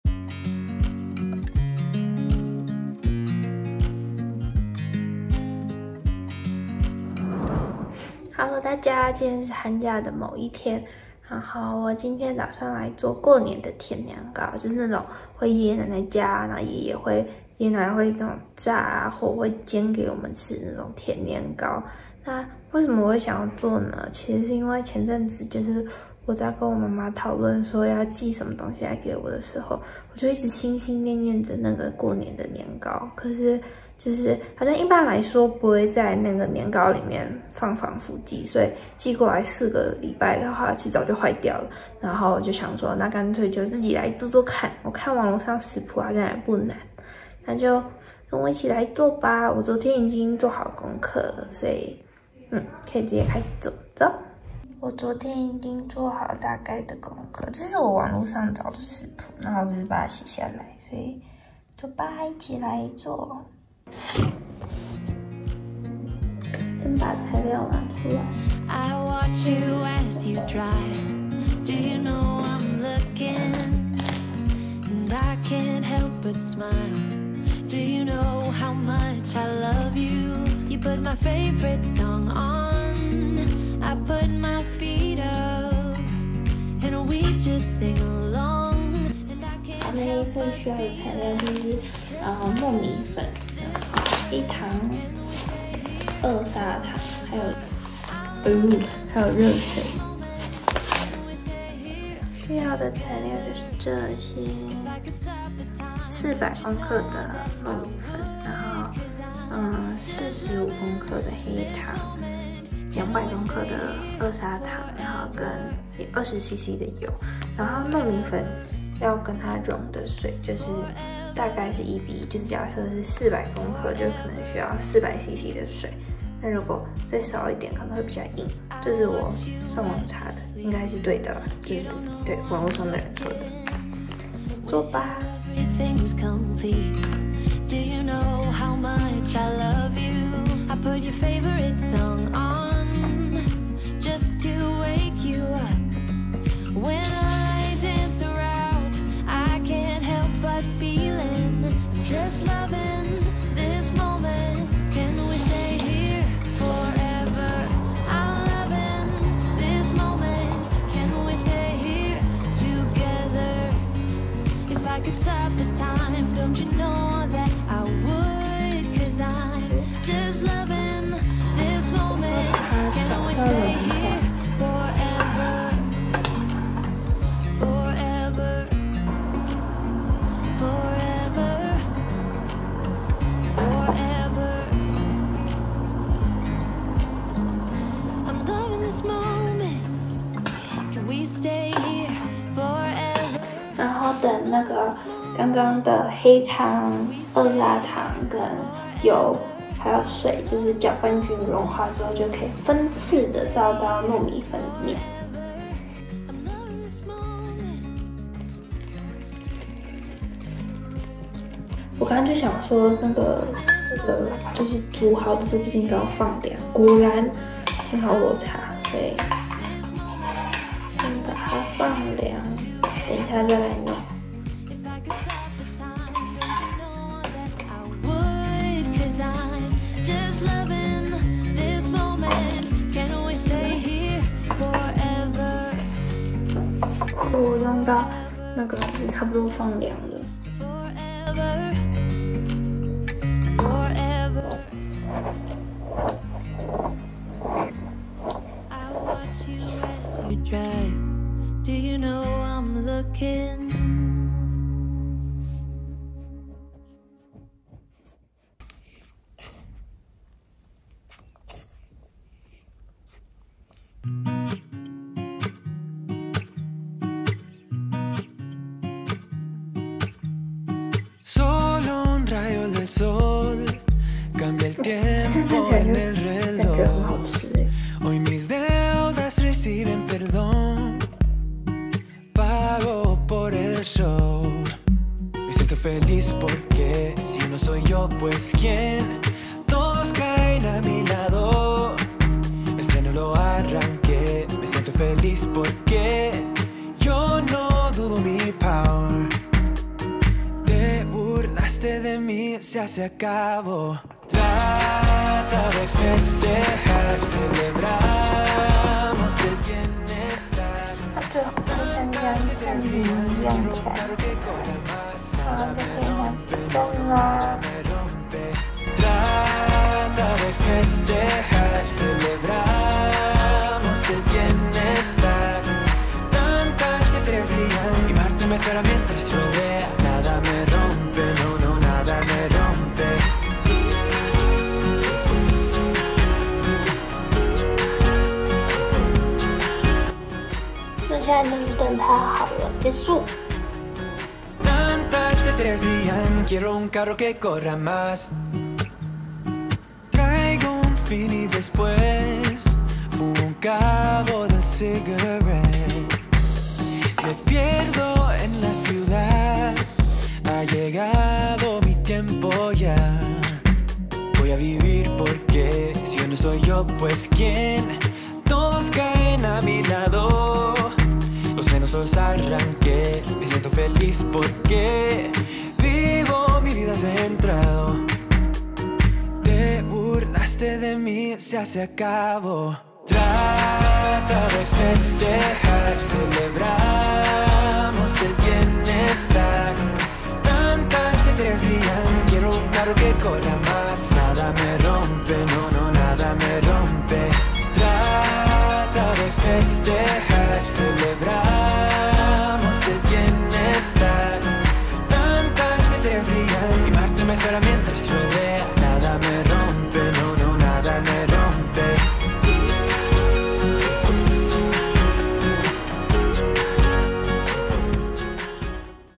西卡的小廚房👩🏻‍🍳│小時候的味道 甜年糕 🏠。農曆新年快樂 🎉
哈囉大家 新年快樂！
前一陣子因為太想念台灣 又想念每年過年回外婆家 都可以吃到的童年味道 甜年糕
所以就查了食譜 想說自己動手做做看～  沒想到超級簡單的！
如果大家也喜歡吃甜年糕的 很推薦可以嘗試看看喔！
而且可以默默發現 原來放的糖那麼多🤣
希望大家喜歡這部影片 那我們下部影片見🙋🏻‍♀️

影片裡 我有稍微減糖 不過真的有點不夠甜 大家可以自己再斟酌～